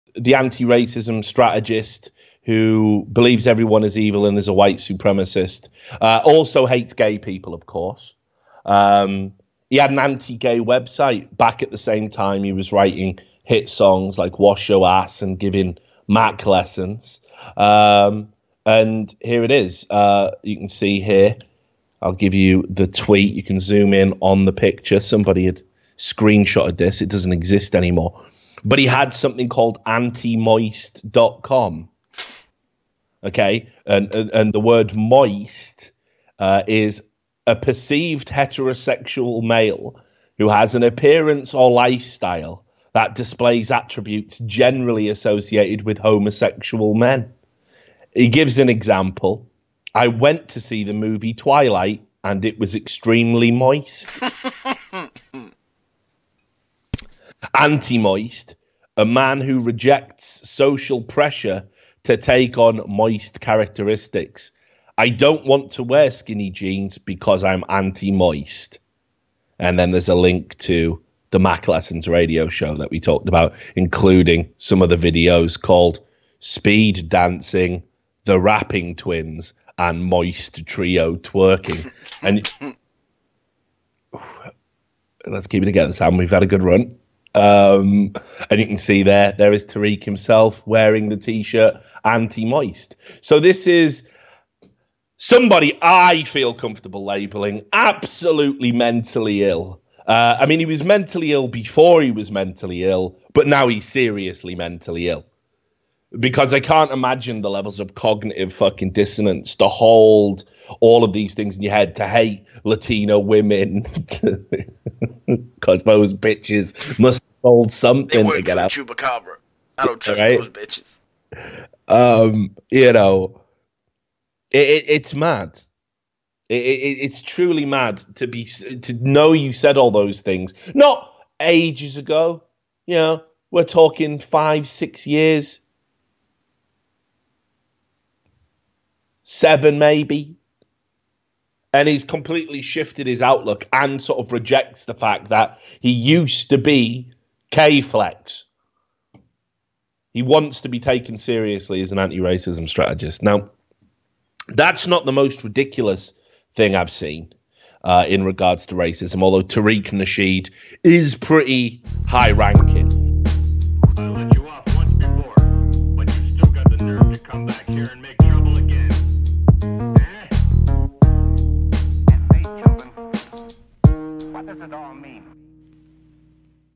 Tariq Nasheed is anti-moist
where Richard and Sam talk about Tariq Nasheed and his old website where he outlines his stance against moistness and moist behavior.